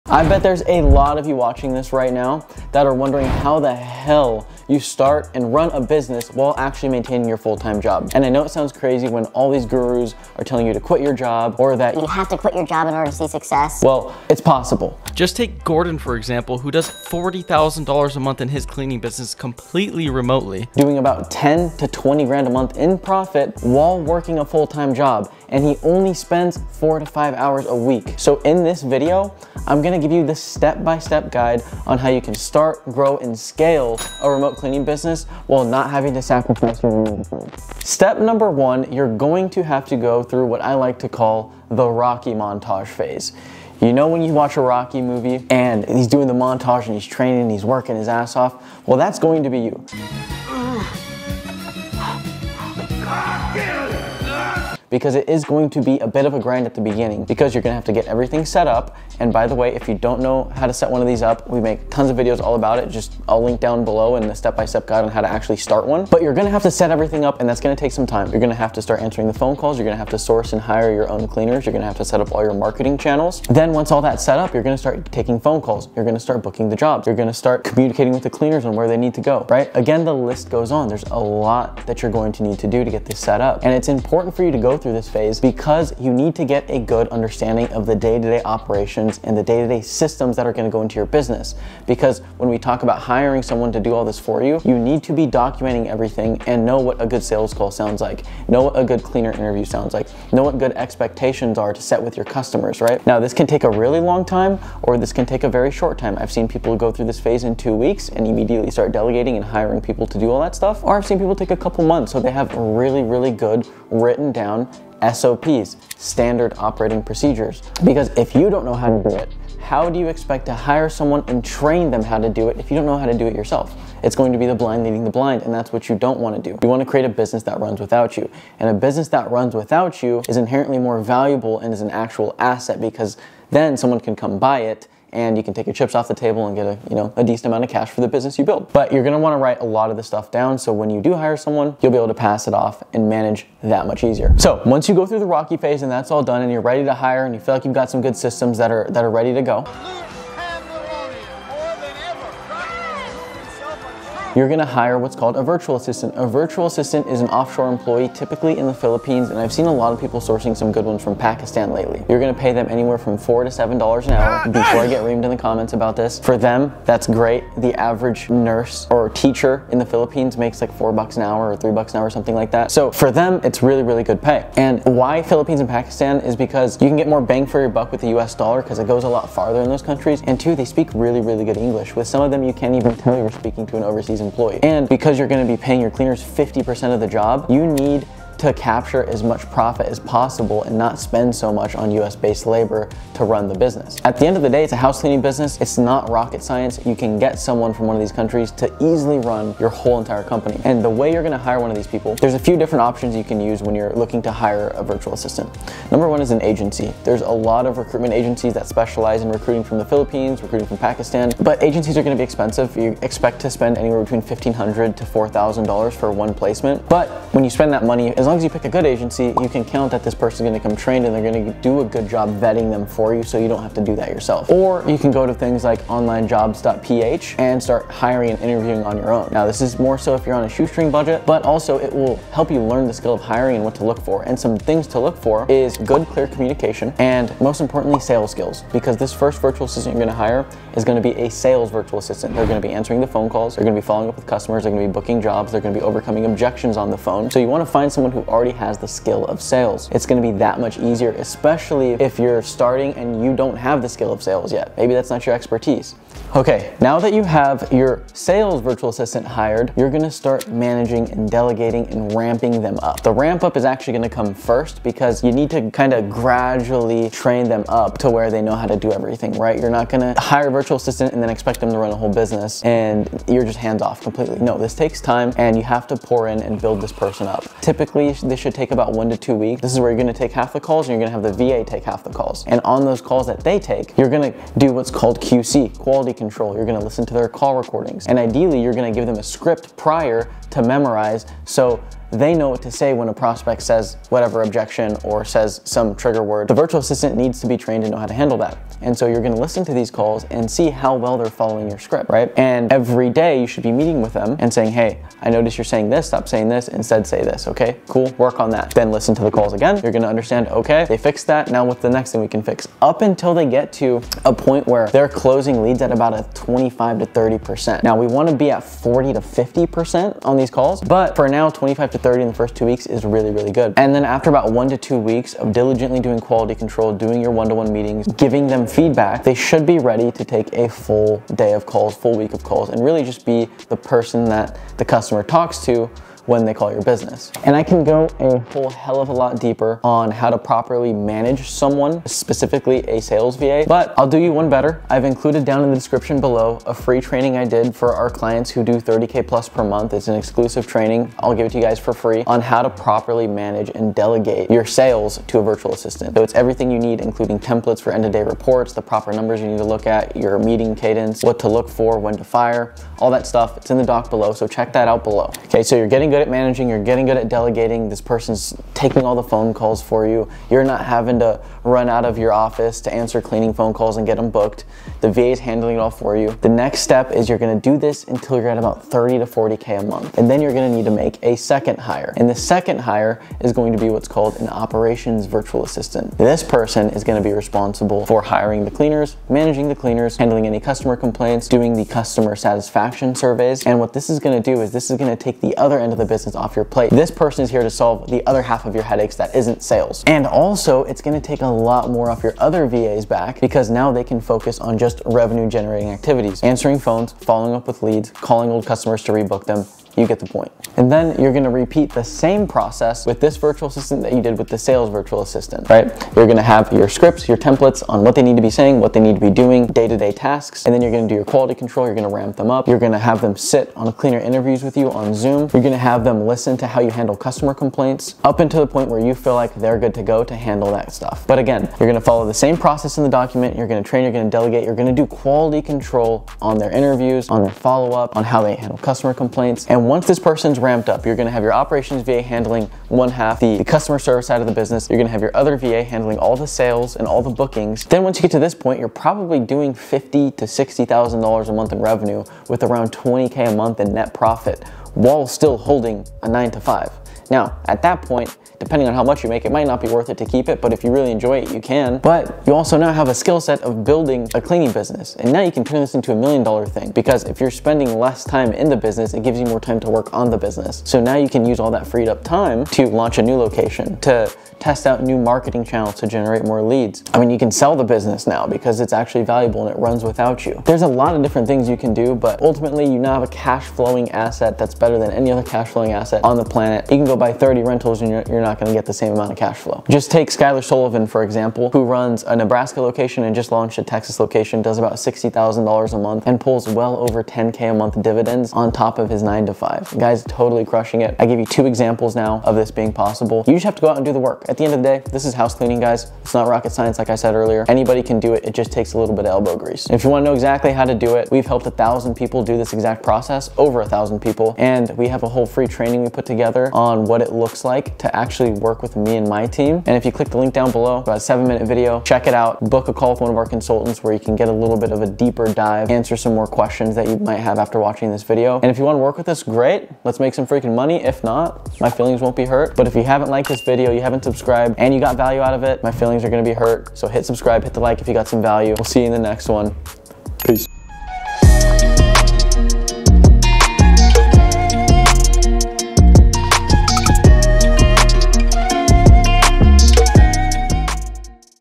How To Start A 6 Figure Remote Cleaning Business While Working Full Time
You don't have to quit your 9-5 job to start and run a successful cleaning business. In fact, if you implement what I tell you in this video then you will only have to spend 5 hours a week running your remote cleaning business because it will be automated. The best part is you will have a cash flowing machine that produces thousands of dollars in profit per month and you may eventually be able to quit your full-time job.

0:00 Intro
0:38 The “Rocky montage”
02:25 Hiring a Virtual assistant
04:16 Training you VA
05:54 Letting the VA work their magic
07:28 Your business is at 20-30k, now what?
08:10 Outro

✅ Learn How to Start A Remote Cleaning Business Side Hustle (Free Training)

👀 We post business advice, Remote Cleaning testimonials and BTS content on our Instagram